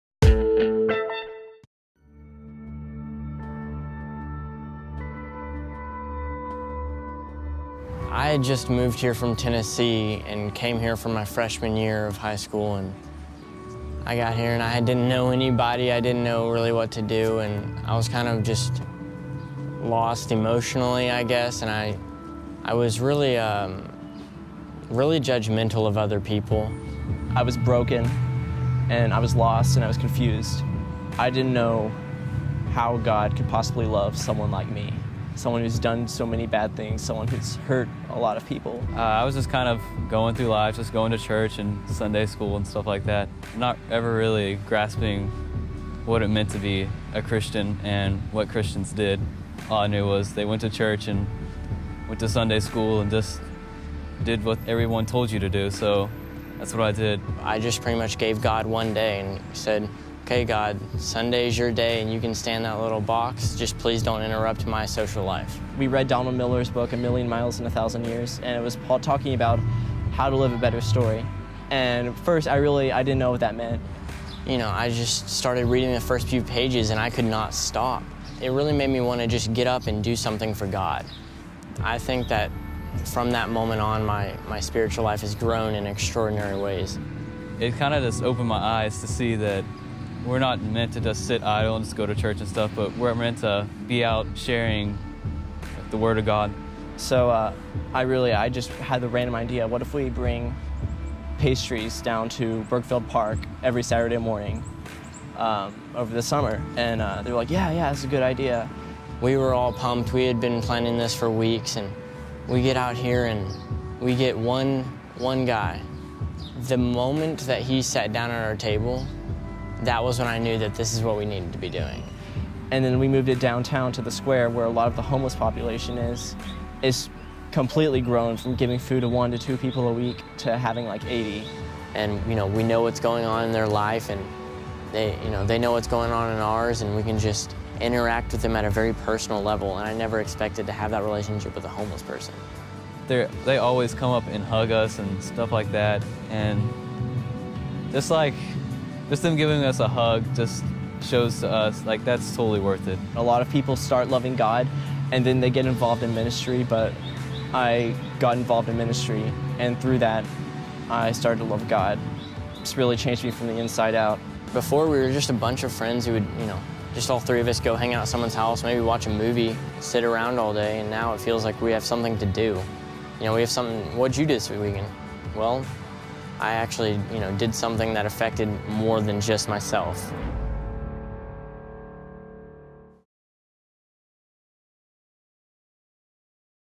Live a Better Story - Bluefish TV
Evan, Jim and Jason were inspired by author, Donald Miller to make their lives count. Instead of looking across the globe, they committed to serving in their own community.